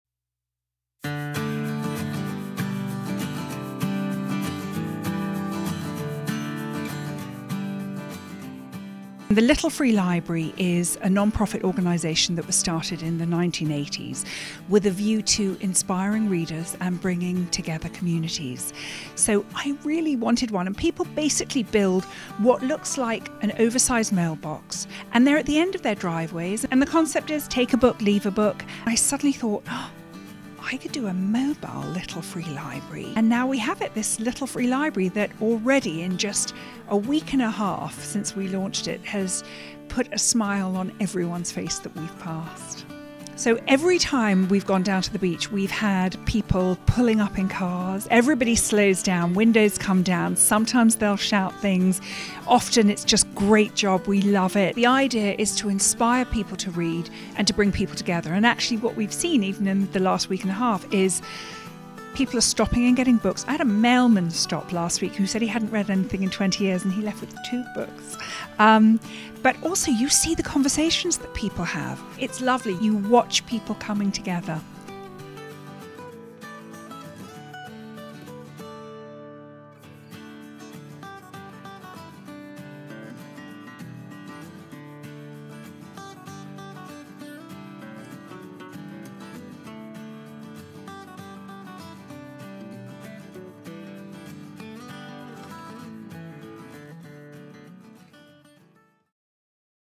Bestest Poll the Planet Series: Celebrity Interview with Jane Green
Jane Green is the author of nineteen novels, including seventeen New York Times Best(est)sellers, one cookbook, and various short stories.

She showed us her Remarkable Bookcycle, a mobile little free library that has been spotted along the beach in Westport, Connecticut.